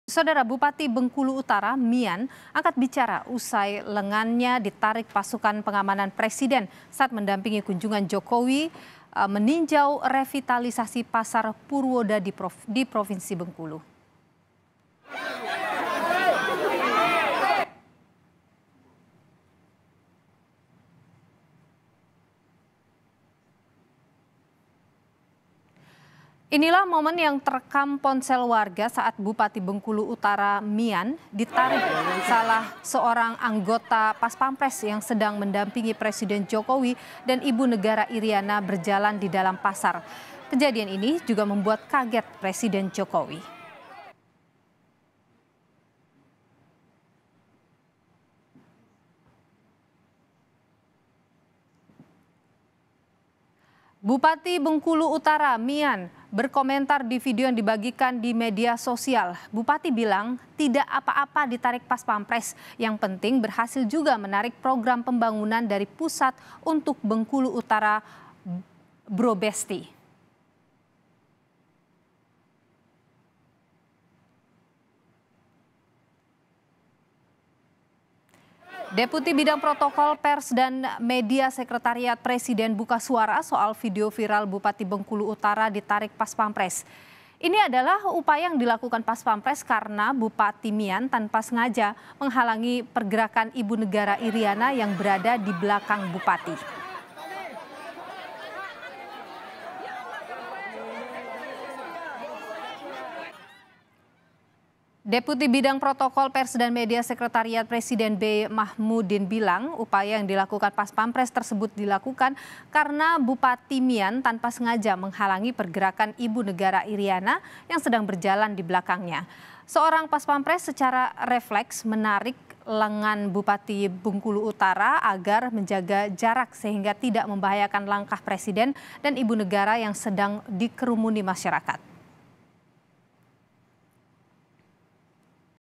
Paspampres Tarik Lengan Bupati Bengkulu, Setpres Buka Suara!
KOMPAS.TV-Paspampres Tarik Lengan Bupati Bengkulu, Setpres Buka Suara!